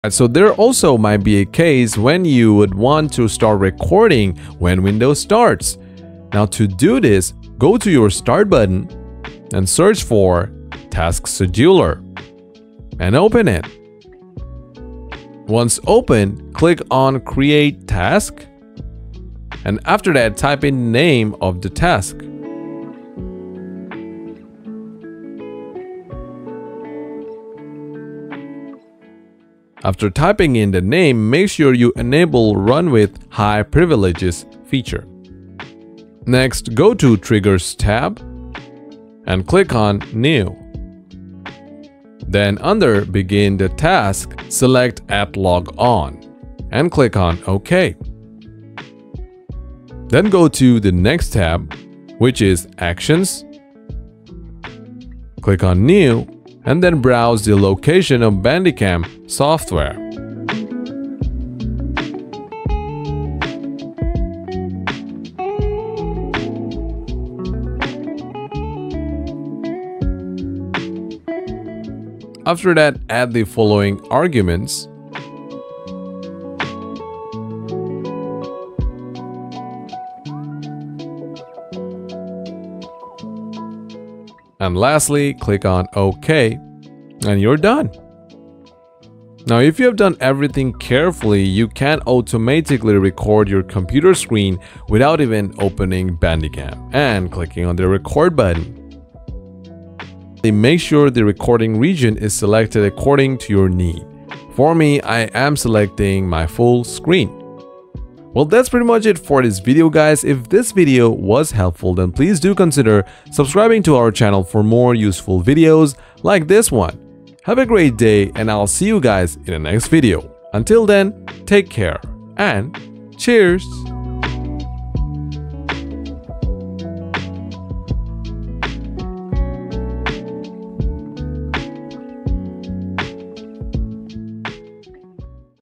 How to start recording automatically with Windows Task Scheduler
If you use the "command-line parameters of Bandicam (/record, /stop, /shutdown)" and "Windows Task Scheduler," you can automatically record your computer screen even when Bandicam is not running. (It’s recommended for advanced users.)

Scheduled Recording with Windows Task Scheduler

1. Click on Start, type in "Task Scheduler," and click "Task Scheduler."
2. [General] tab: Click "Create Task..." and type in the name of task such as "Bandicam start recording." Then, check "Run with highest privileges."
3. [Triggers] tab: Enter the desired start time of the recording ("On a schedule" or "At log on").
4. [Actions] tab: Click "Browse..." and select the executable file (C:\Program Files\Bandicam\bdcam.exe). Then, enter "/record /nosplash" in "Add arguments."
5. You have now created a Scheduled Task to start recording automatically.